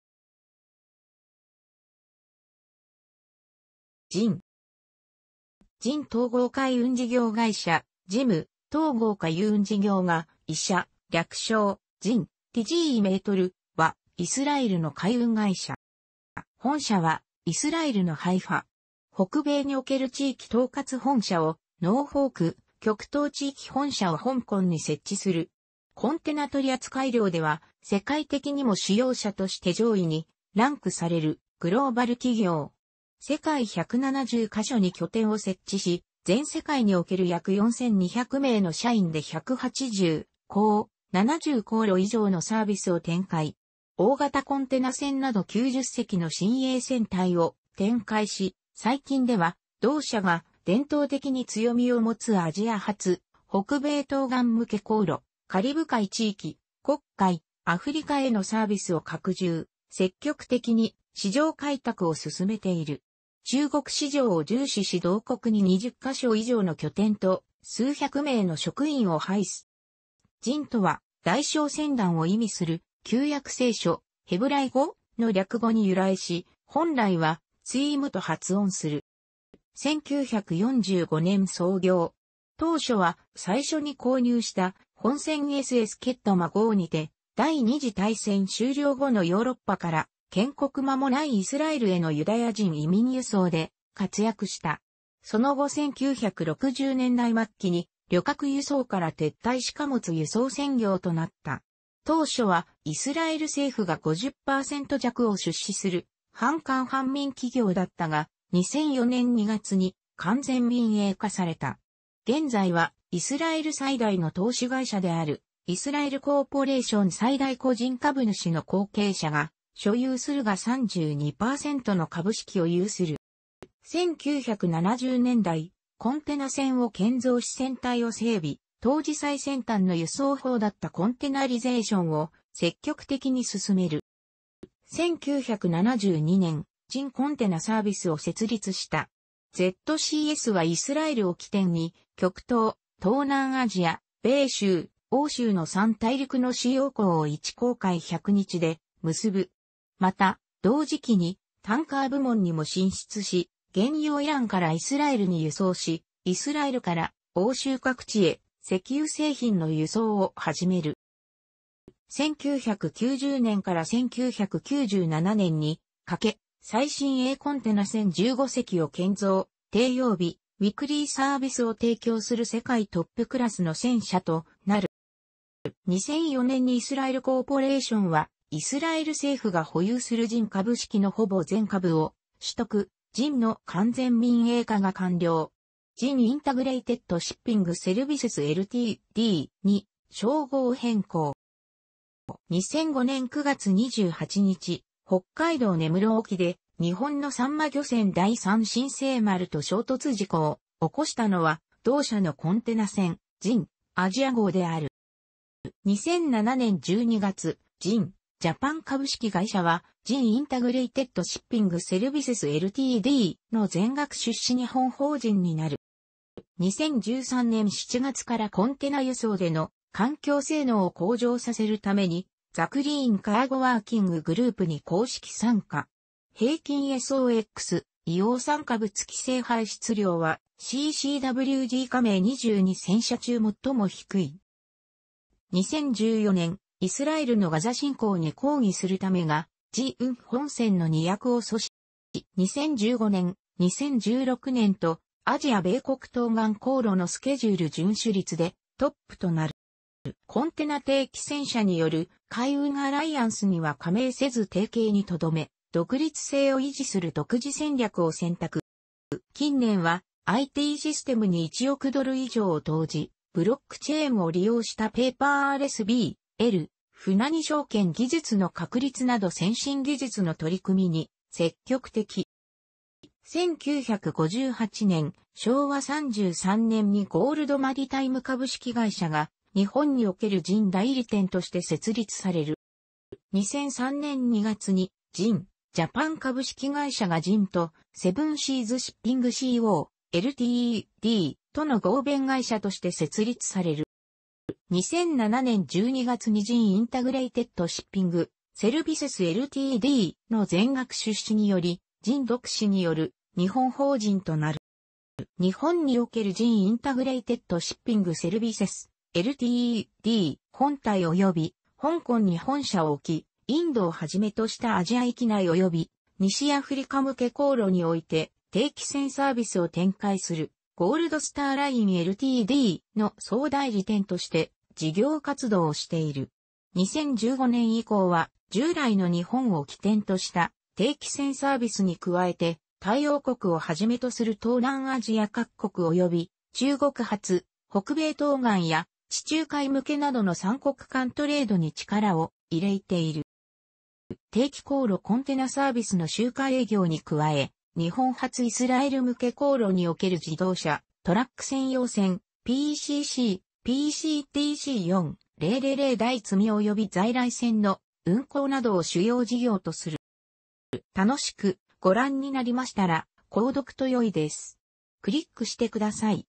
ZIM
ZIM統合海運事業会社（ジムとうごうかいうんじぎょうがいしゃ、略称: ZIM、, "tzī"m", ）は、イスラエルの海運会社。

本社はイスラエルのハイファ。北米における地域統括本社をノーフォーク、極東地域本社を香港に設置する。コンテナ取扱量では世界的にも主要社として上位にランクされるグローバル企業。世界170カ所に拠点を設置し、全世界における約4,200名の社員で180港、70航路以上のサービスを展開。大型コンテナ船など90隻の新鋭船隊を展開し、最近では、同社が伝統的に強みを持つアジア発北米東岸向け航路、カリブ海地域、黒海、アフリカへのサービスを拡充、積極的に市場開拓を進めている。中国市場を重視し同国に20ヶ所以上の拠点と数100名の職員を配す。

ZIM とは 「大商船団」 を意味する旧約聖書ヘブライ語 「צִי יַמִּי מִסְחָרִי (tzī yammī mischārī)」の略語に由来し、本来は 「ツィーム (tzīm)」 と発音する。

1945年創業。当初は最初に購入した本船 SS Kedma 号にて第二次大戦終了後のヨーロッパから建国間もないイスラエルへのユダヤ人移民輸送で活躍した。その後1960年代末期に旅客輸送から撤退し貨物輸送専業となった。当初はイスラエル政府が 50%弱を出資する半官半民企業だったが、2004年2月に完全民営化された。現在はイスラエル最大の投資会社であるイスラエル・コーポレーション最大個人株主の後継者が所有する が32％の株式を有する。

1970年代、コンテナ船を建造し船隊を整備、当時最先端の輸送方だったコンテナリゼーションを積極的に進める。

1972年、ZIM Container Service (ZCS)を設立した。ZCSはイスラエルを起点に、極東、東南アジア、米州、欧州の3大陸の主要港を1航海100日で結ぶ。また、同時期にタンカー部門にも進出し、原油をイランからイスラエルに輸送し、イスラエルから欧州各地へ石油製品の輸送を始める。

1990年～1997年にかけ、最新鋭コンテナ船15隻を建造、定曜日ウィクリーサービスを提供する世界トップクラスの船社となる。

2004年にイスラエル・コーポレーションはイスラエル政府が保有するZIM株式のほぼ全株を取得、ZIMの完全民営化が完了。ZIM Integrated Shipping Services Ltd. に商号変更。

2005年9月28日、北海道根室沖で日本のサンマ漁船 第三新生丸と衝突事故を起こしたのは、同社のコンテナ船「ZIMアジア号」 (ZIM Asia) である。

2007年12月、ZIMジャパン株式会社はZIM Integrated Shipping Services Ltd.の全額出資日本法人になる。

2013年7月からコンテナ輸送での環境性能を向上させるために、The Clean Cargo Working Group (CCWG)に公式参加。平均SOX（硫黄酸化物）規制排出量はCCWG加盟22船社中最も低い。

2014年、イスラエルのガザ侵攻に抗議するためがZIM本船の荷役を阻止。

2015年、2016年とアジア〜米国東岸航路のスケジュール遵守率でトップとなる。

コンテナ定期船社による海運アライアンスには加盟せず提携にとどめ独立性を維持する独自戦略を選択。

近年はITシステムに１億ドル以上を投じ、ブロックチェーンを利用したペーパーレスB/L（船荷証券）技術の確立など先進技術の取組みに積極的。

1958年（昭和33年）にゴールドマリタイム株式会社が日本におけるZIM 代理店として設立される。

2003年２月にZIMジャパン株式会社がZIMと Seven Seas Shipping Co., Ltd.との合弁会社として設立される。

2007年12月にZIM Integrated Shipping Services Ltd.の全額出資によりZIM独資による日本法人となる。

日本におけるZIM Integrated Shipping Services ltd. 本体および、香港に本社を置きインドを始めとしたアジア域内および西アフリカ向け航路において定期船サービスを展開する Gold Star Line Ltd. (GSL)の総代理店として事業活動をしている。2015年以降は、従来の日本を基点とした定期船サービスに加えて、タイ王国をはじめと...